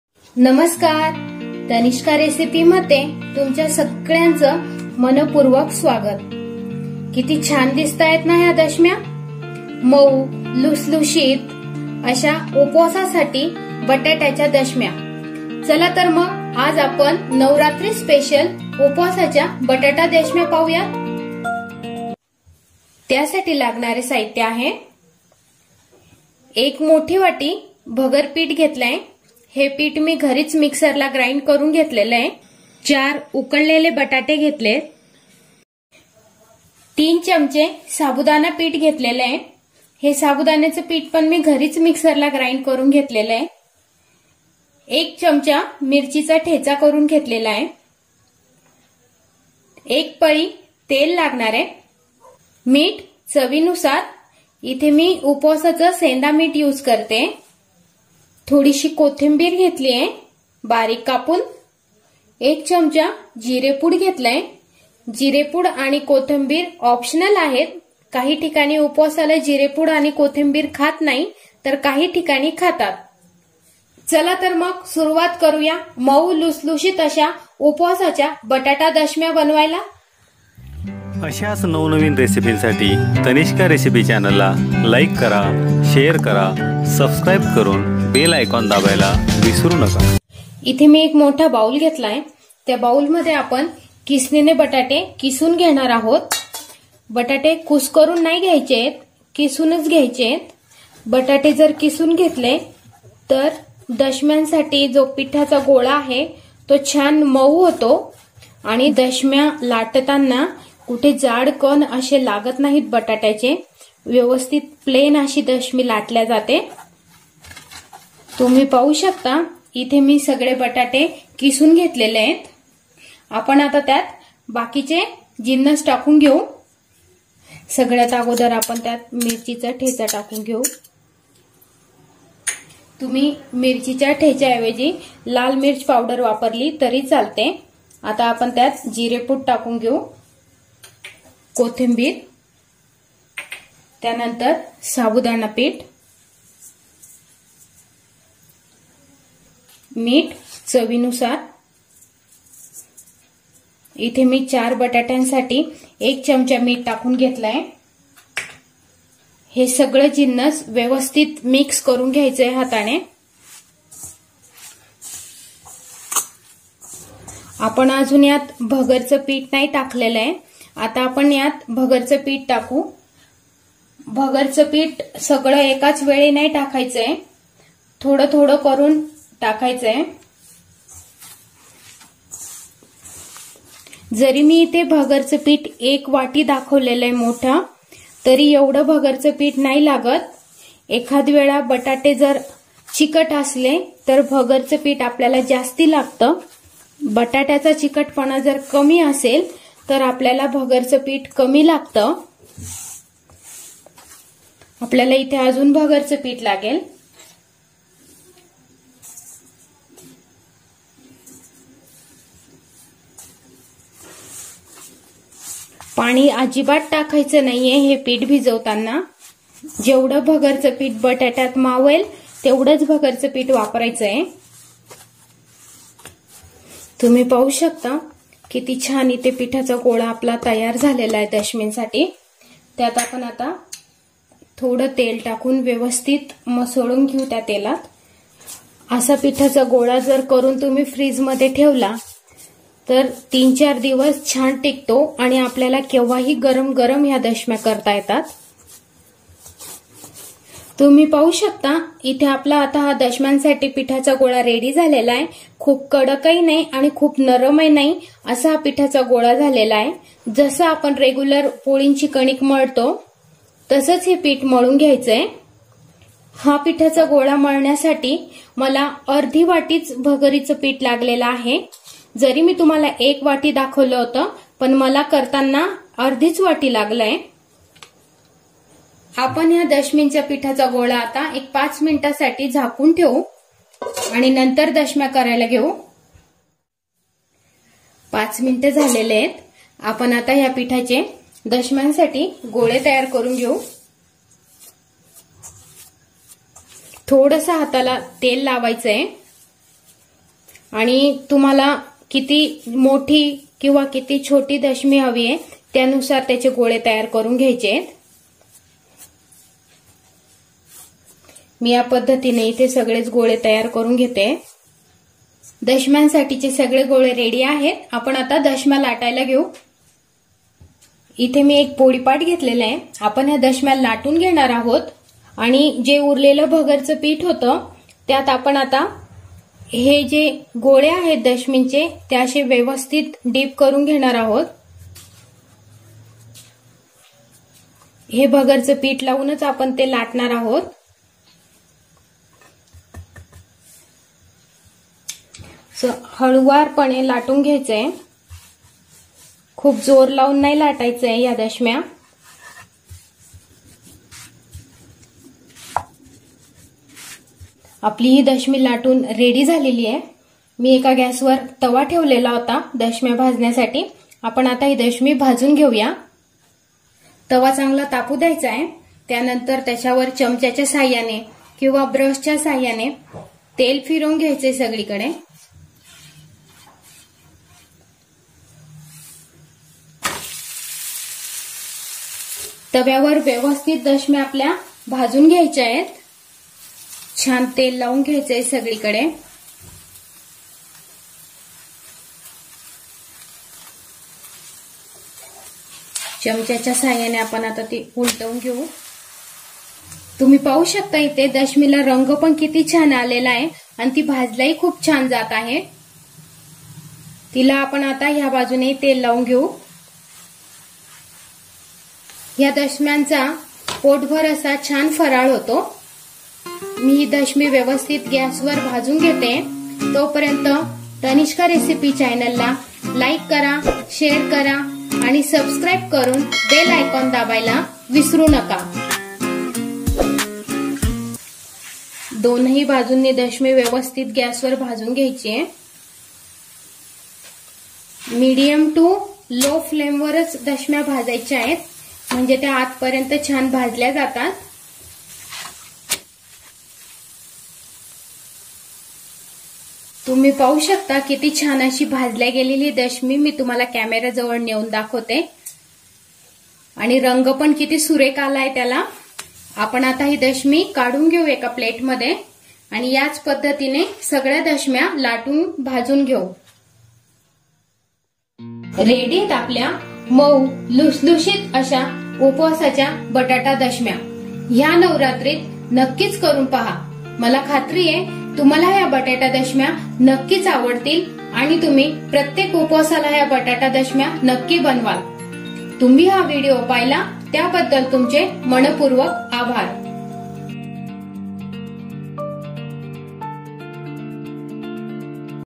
मऊ आणि लुसलुशीत उपवासाच्या बटाटा दशम्या | UPVASACHYA DASHMYA | upvasachi Batata dashami |दशमीची पोळी
Materials required for upwas batata dashmi of fasting-
1 large bowl of bhagar flour
4 potatoes, peeled
3 tablespoons of sago flour
1 tsp green chili powder
1 large spoon of oil
Salt to taste
 A little coriander
1 tsp cumin powder

उपवासाच्या बटाटा दशम्या (upwas batata dashmi) करण्यासाठी लागणारे साहित्य-
१ मोठी वाटी भगर पीठ
४ बटाटे उकडून घेतलेले,
३ चमचे साबुदाणा पीठ
१ चमचा हिरव्या मिरचीचा ठेचा
१ मोठी पळी तेल
मीठ चवीनुसार
थोडीशी कोथंबीर
१ चमचा जिरे पूड

उपवास काळात पोटभर जेवणासाठी खास उपवासाची पुरी भाजी | upvas puri bhaji |उपवास पुरी भाजी | puri bhaji

अशाच चवदार रेसीपीसाठी Tanishqa recipe च्या खालील लिंकवर क्लिक करा.